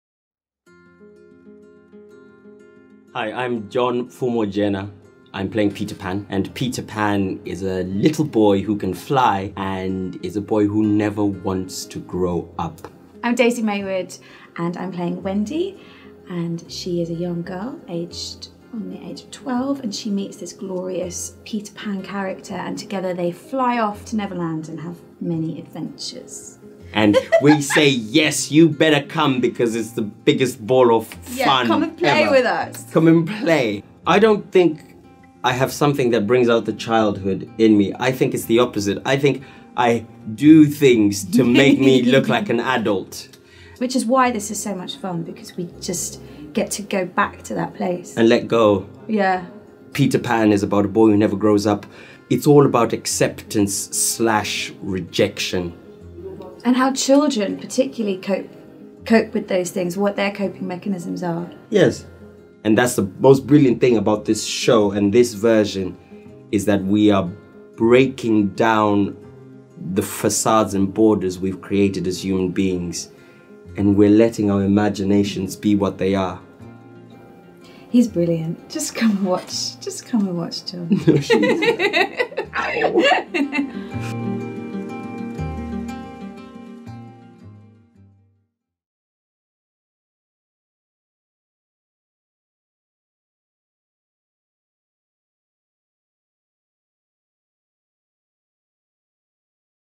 Interview with Peter Pan and Wendy
Enjoy an exclusive interview with John Pfumojena, who plays Peter Pan and Daisy Maywood who plays Wendy Darling,

Following a sold out run at the Bristol Old Vic, and a reimagining at the National Theatre in 2016, Cookson’s brilliantly inventive Peter Pan returns to the capital city. The show features spectacular aerial acrobatics, live music from an on-stage band – and no shortage of magic and mischief.